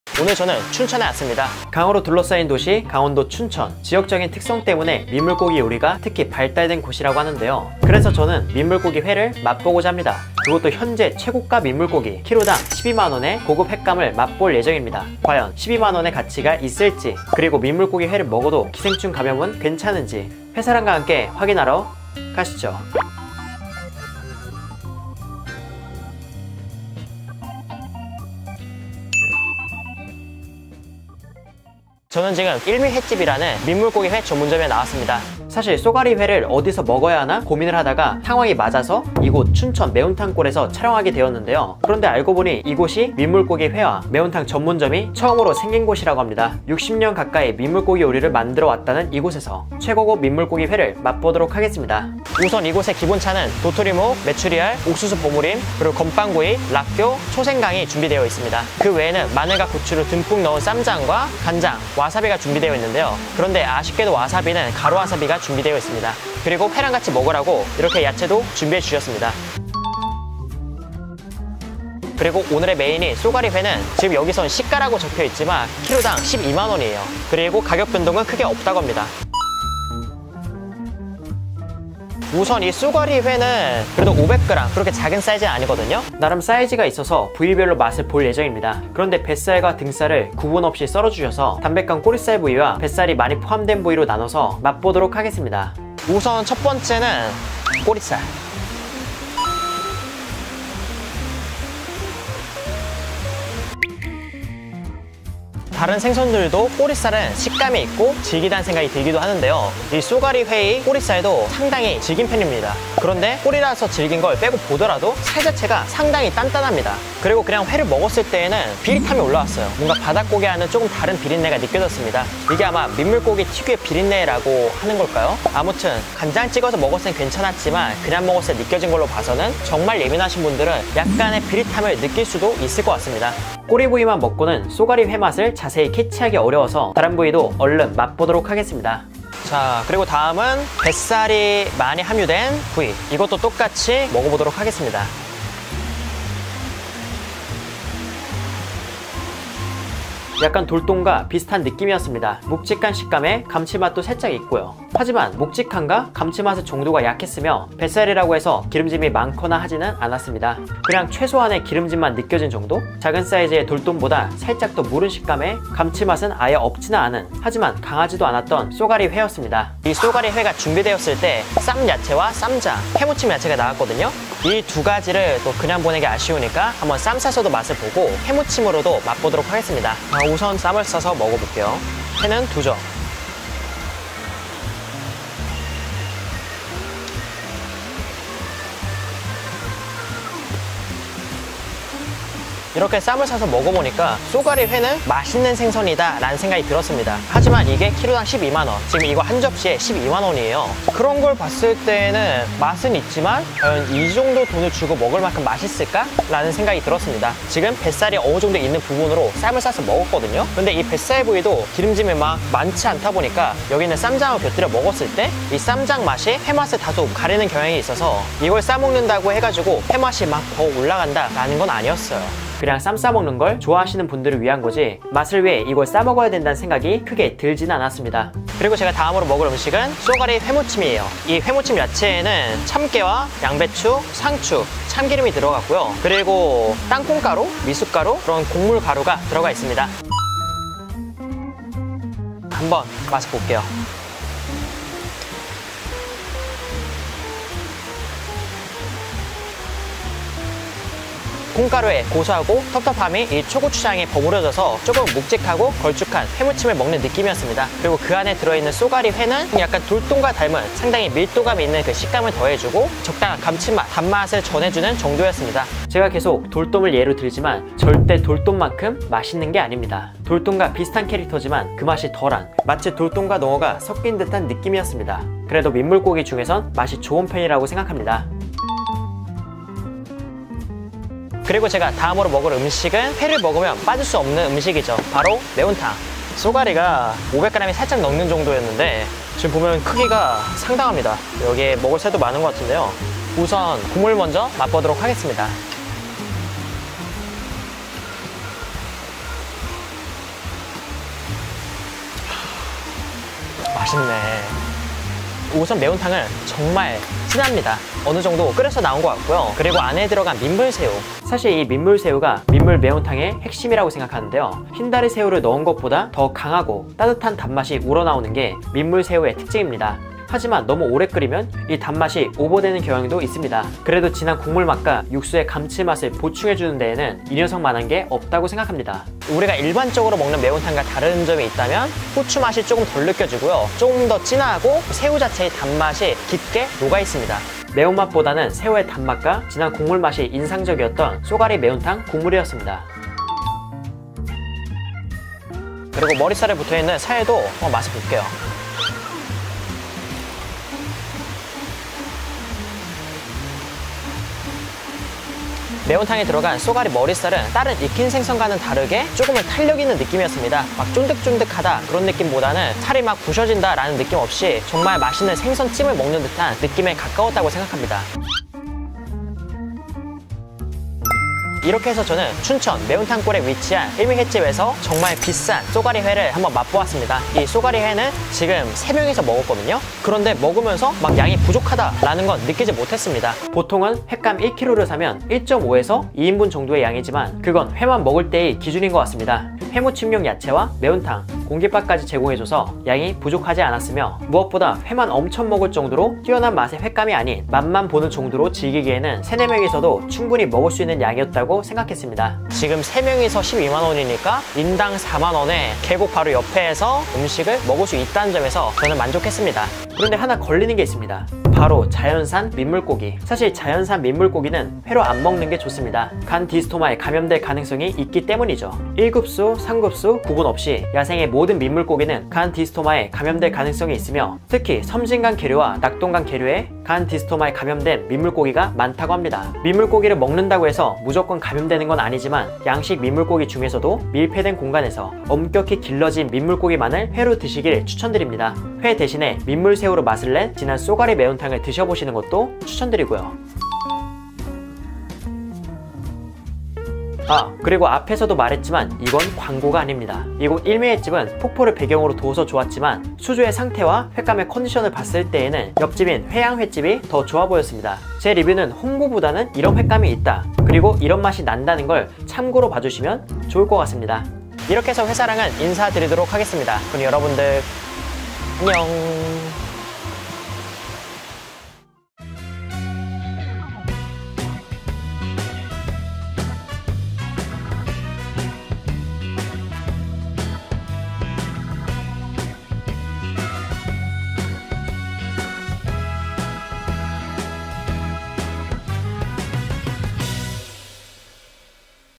kg당 12만원짜리 민물고기를 회로 맛보았습니다!(feat.민물고기 날로 먹어도 괜찮을까?)
안녕하세요 회사랑입니다!
오늘 저는 예전부터 궁금했던 쏘가리 회를 맛보았는데요

철갑상어와 거의 맘 먹는
어떤 날에는 철갑상어 보다 비싼 쏘가리!

과연 얼마나 맛있길래 이렇게 비싼지
회사랑이 한번 맛보도록 하겠습니다.

그럼 같이 보시죠 :-)

*식당정보
- 식당명 : 일미횟집
- 식당 위치 : 강원 춘천시 서면 삿갓봉길 23
- 영업시간 : 11:00-21:00(영업시간 변동 가능성 있으므로 전화로 확인 필수)
- 리뷰 메뉴 : 쏘가리 1kg (쏘가리회, 회무침 야채, 매운탕, 밥/약 3-4인분)
- 특이사항 : 민물고기 요리의 원조집이라고 볼 수 있음.